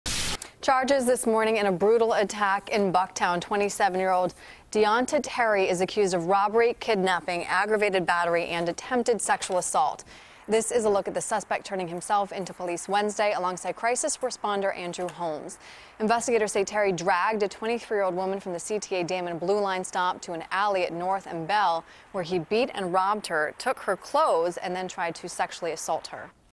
Man Charged With Beating, Robbing Woman In Bucktown
CBS 2's Erin Kennedy reports.